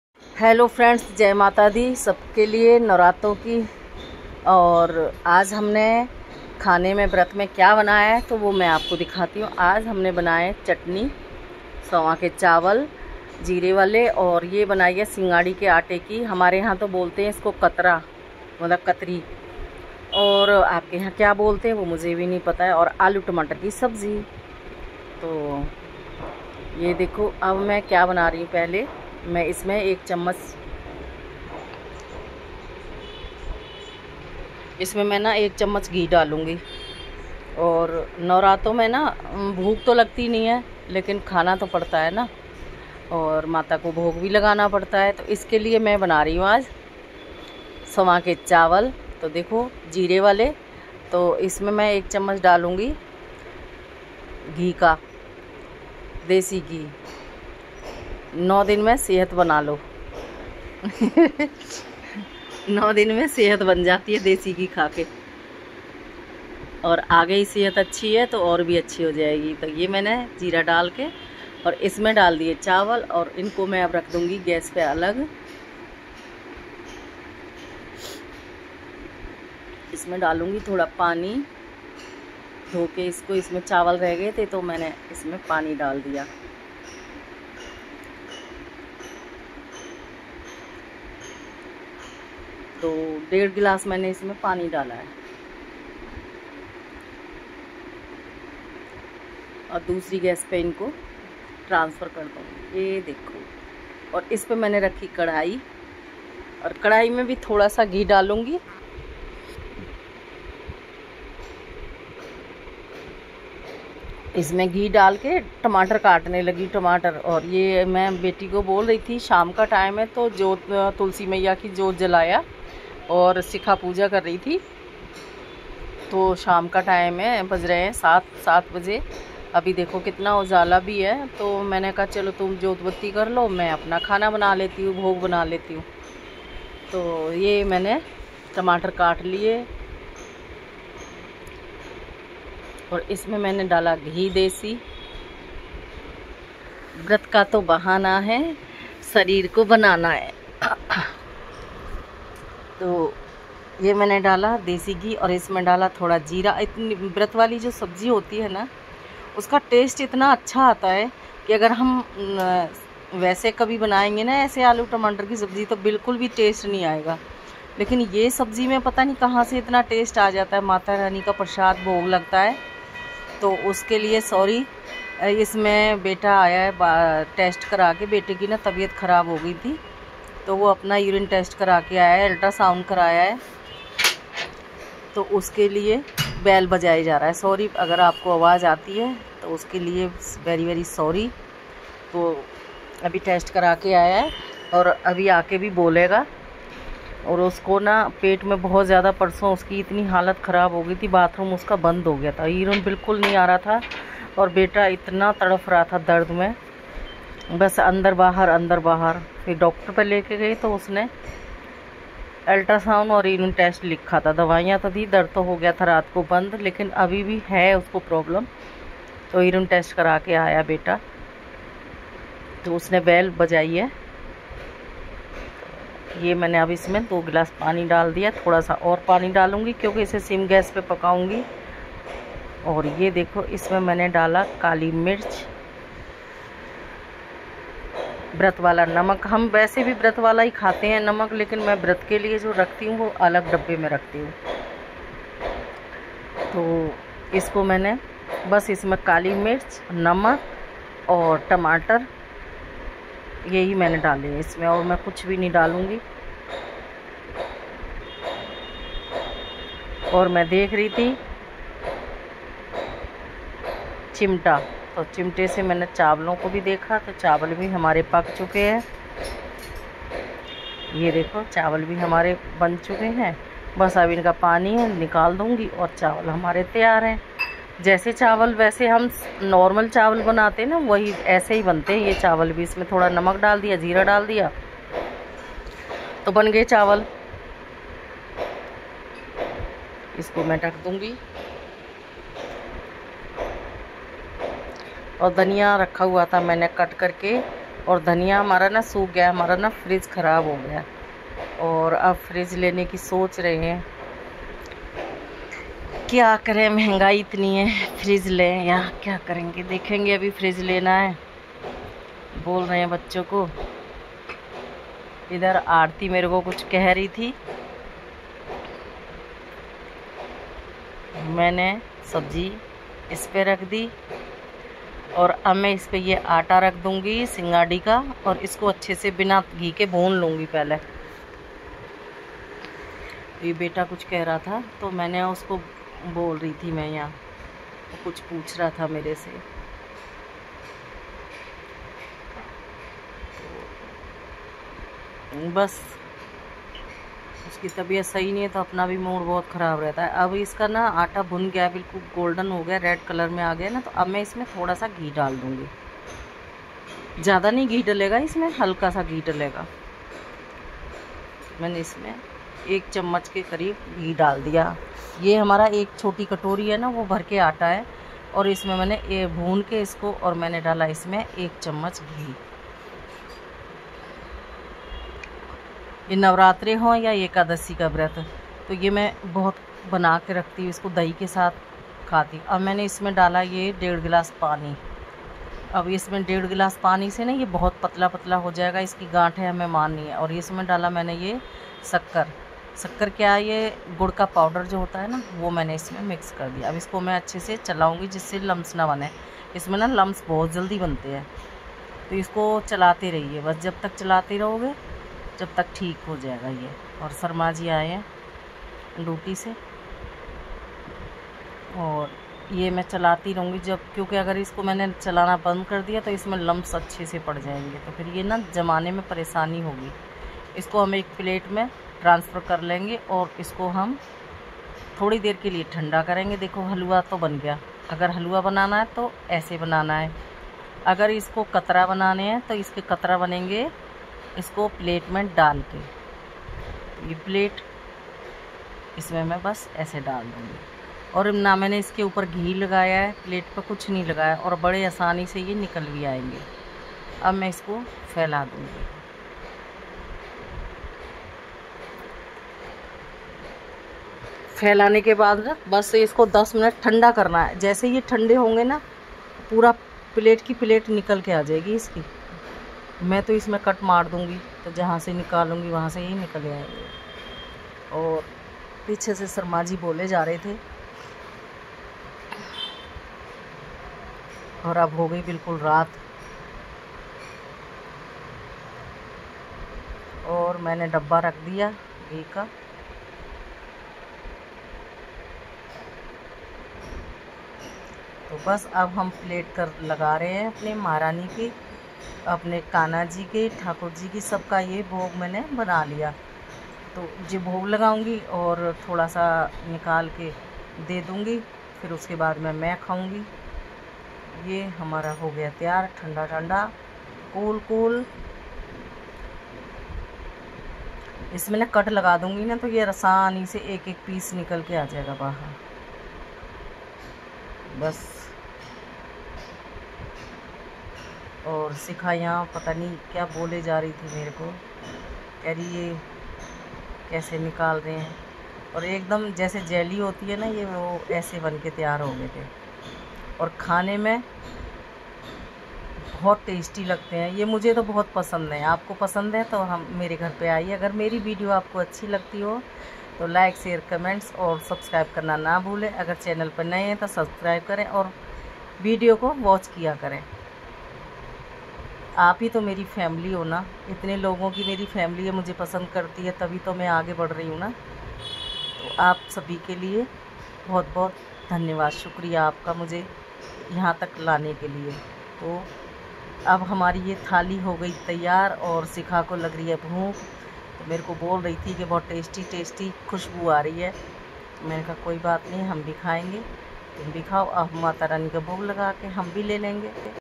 आज हमने बनाई सिंगड़ी के कटरे और आलू की सब्जी और समा के चावल vrat thali
Aaj humne banae Sama ke chawal aalu ki sabji aur singari ke Katra#homemade recipe healthy youtube food video maggie aaj # singari ki recipe singari ke ktra kase banaye madhav ki rasoi recipe